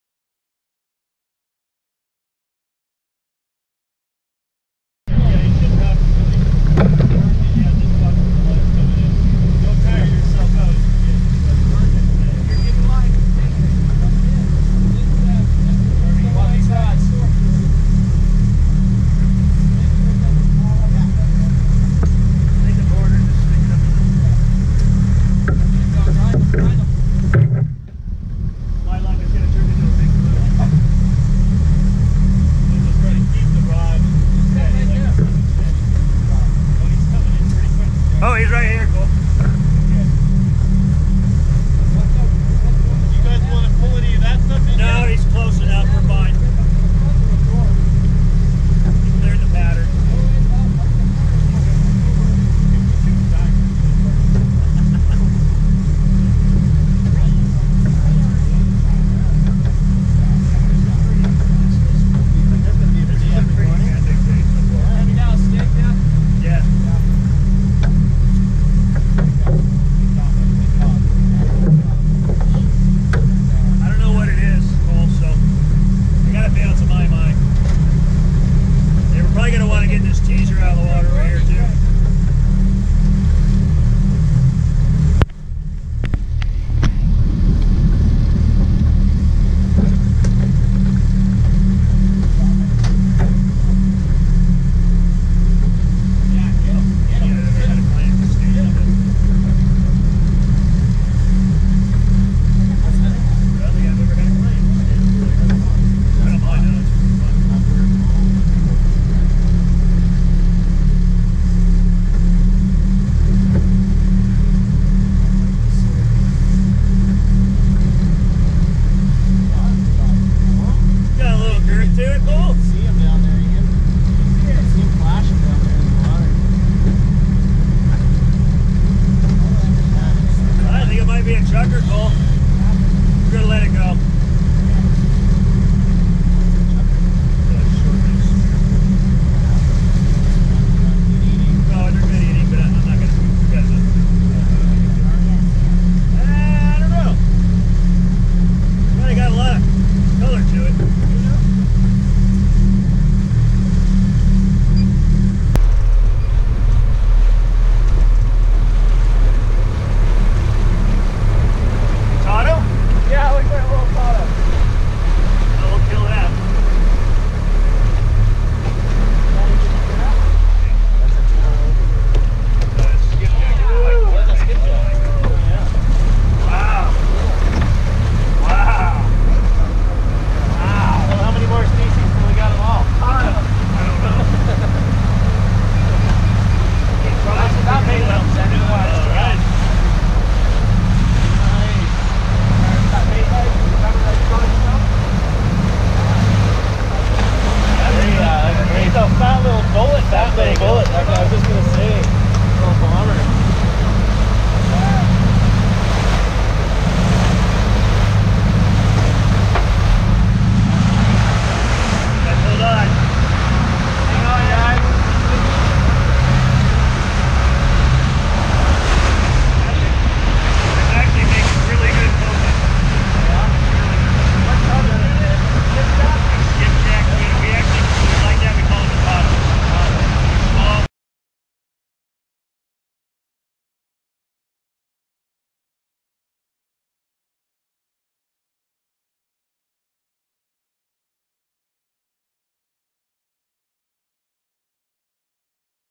Hula Girl Sport Fishing Charters in Kona Hawaii
8#  Aku (Skipjack Tuna) caught on February 25, 2019